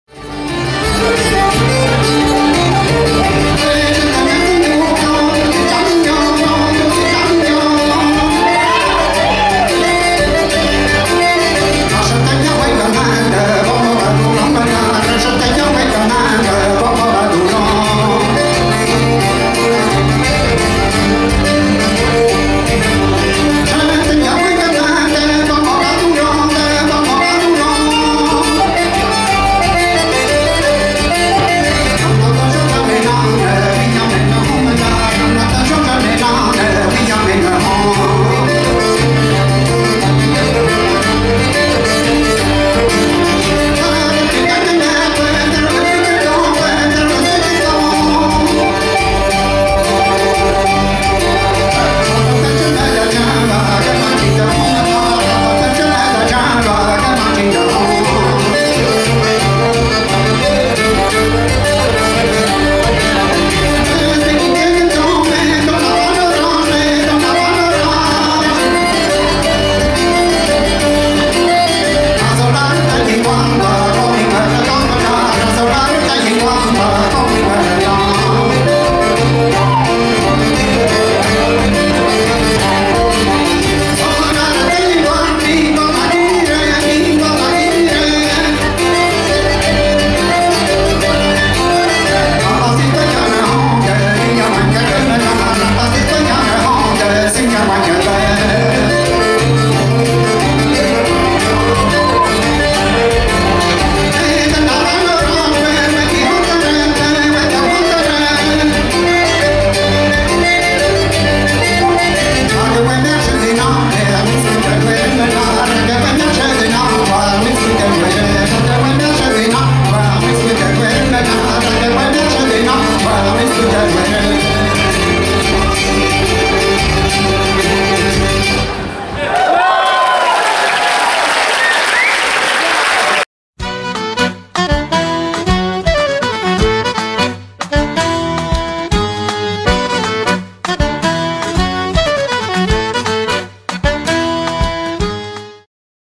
Festival Interceltique / YANN FAÑCH KEMENER TRIO
Yann Fañch Kemener Trio au Festival Interceltique de Lorient 2017.

Crédit Vidéo : (cc) Norbert Fest Noz